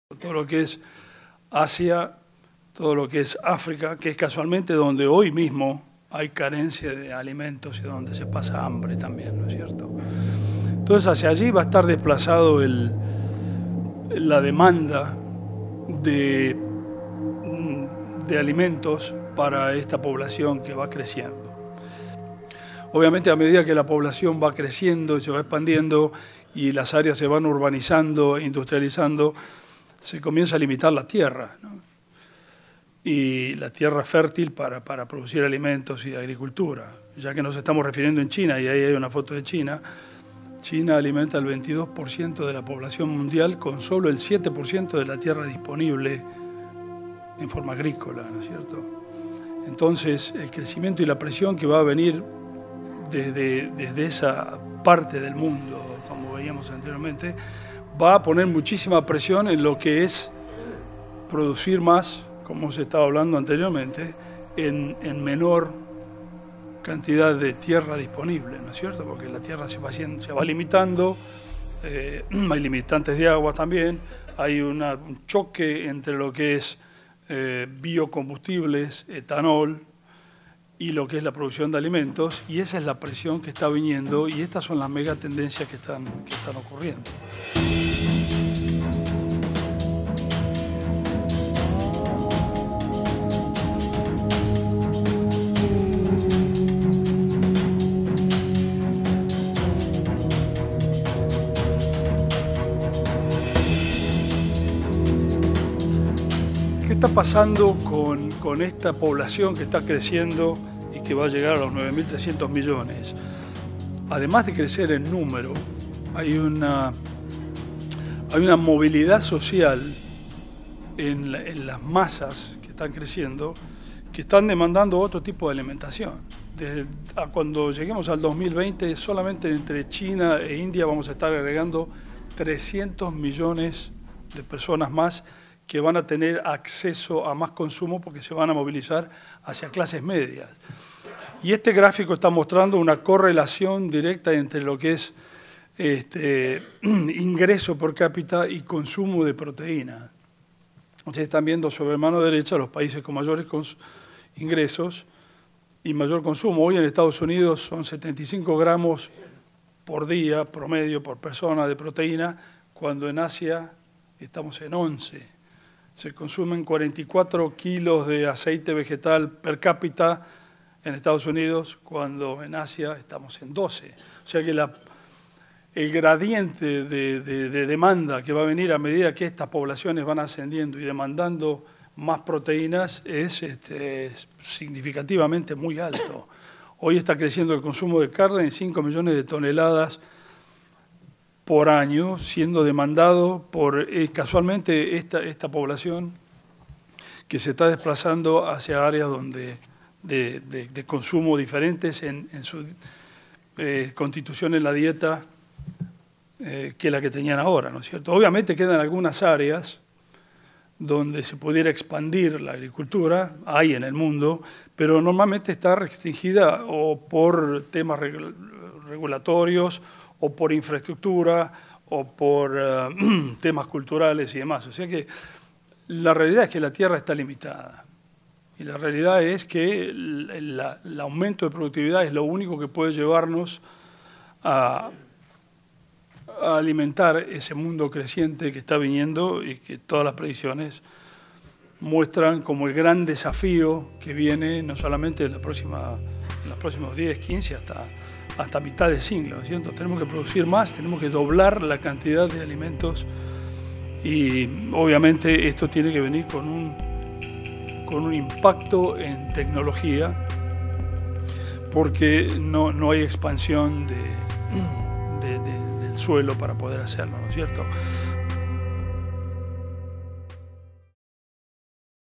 China alimenta al 22% de la población mundial con solo el 7% de la tierra disponible
Jorge La Roza, Presidente de Dow Región Sur, estuvo presente en el 2º Encuentro de Reflexión Argentina Visión 2020, donde analizó las posibilidades del agro argentino de cara al 2050 y a la creciente demanda mundial de alimentos, en particular proveniente de Asia y Africa.